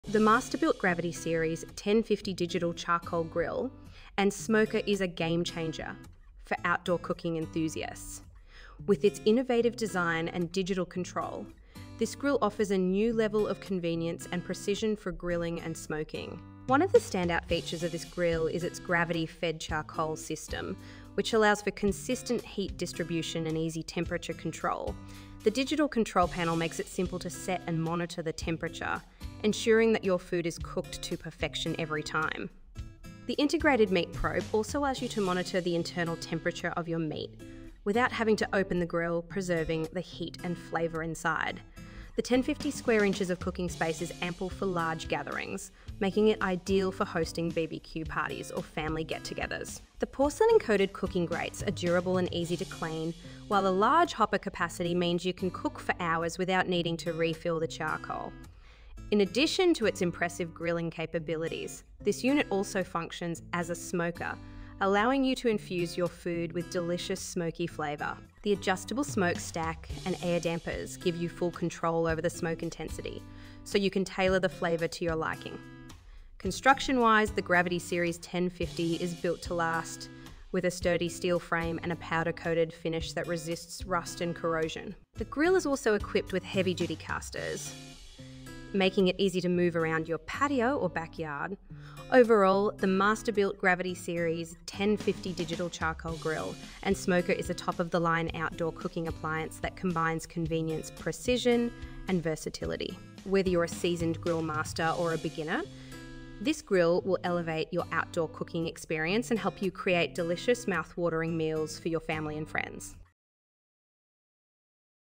Masterbuilt® Gravity Series® 1050 Digital Charcoal Grill and Smoker with Digital Control Review
The Masterbuilt Gravity Series 1050 Digital Charcoal Grill and Smoker is a game-changer for outdoor cooking enthusiasts. With its innovative design and digital control, this grill offers a new level of convenience and precision for grilling and smoking.

One of the standout features of this grill is its gravity-fed charcoal system, which allows for consistent heat distribution and easy temperature control. The digital control panel makes it simple to set and monitor the temperature, ensuring that your food is cooked to perfection every time. The integrated meat probe also allows you to monitor the internal temperature of your meat without having to open the grill, preserving the heat and flavor inside.

The 1050 square inches of cooking space is ample for large gatherings, making it ideal for hosting BBQ parties or family get-togethers. The porcelain-coated cooking grates are durable and easy to clean, while the large hopper capacity means you can cook for hours without needing to refill the charcoal.

In addition to its impressive grilling capabilities, this unit also functions as a smoker, allowing you to infuse your food with delicious smoky flavor. The adjustable smoke stack and air dampers give you full control over the smoke intensity, so you can tailor the flavor to your liking.

Construction-wise, the Gravity Series 1050 is built to last, with a sturdy steel frame and a powder-coated finish that resists rust and corrosion. The grill is also equipped with heavy-duty casters, making it easy to move around your patio or backyard.

Overall, the Masterbuilt Gravity Series 1050 Digital Charcoal Grill and Smoker is a top-of-the-line outdoor cooking appliance that combines convenience, precision, and versatility. Whether you're a seasoned grill master or a beginner, this grill will elevate your outdoor cooking experience and help you create delicious, mouth-watering meals for your family and friends.